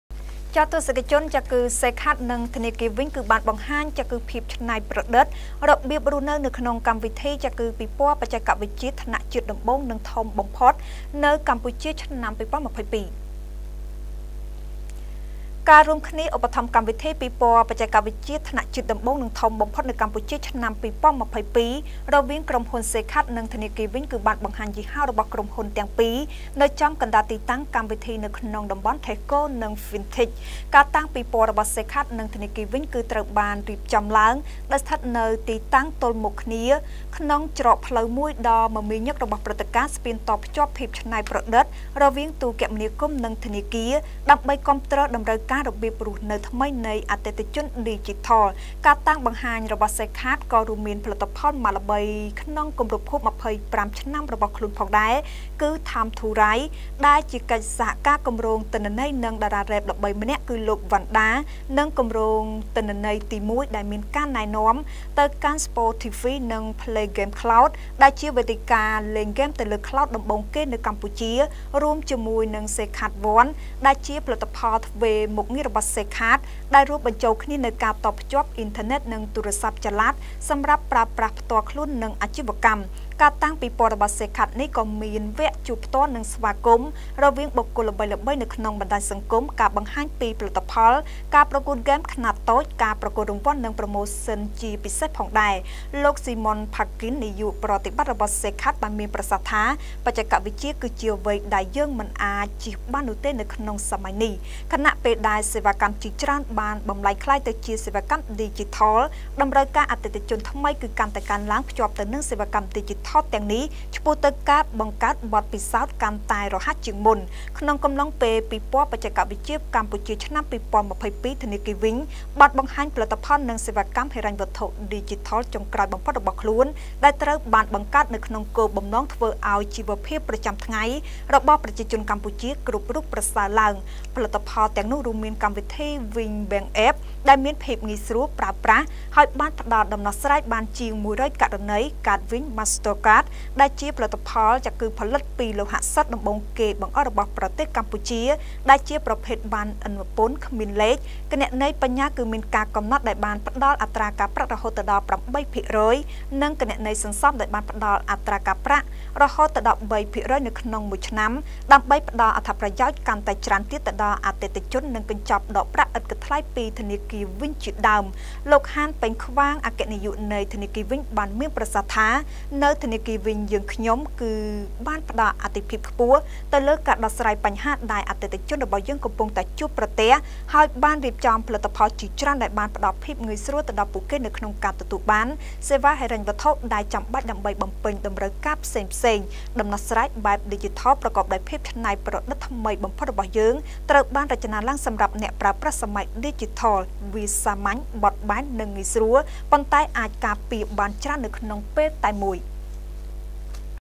សែលកាត និង ធនាគារវីង បង្ហាញពីភាពច្នៃប្រតិឌរបៀបរស់នៅក្នុងកម្មវិធីពិពរណ៍បច្ចេកវិទ្យាថ្នាក់ជាតិ
សែលកាត និង ធនាគារ វីង បង្ហាញពីភាពច្នៃប្រតិឌរបៀបរស់នៅក្នុងកម្មវិធីពិពរណ៍បច្ចេកវិទ្យថ្នាក់ជាតិដំបូង និងធំបំផុតនៅកម្ពុជាឆ្នាំ២០២២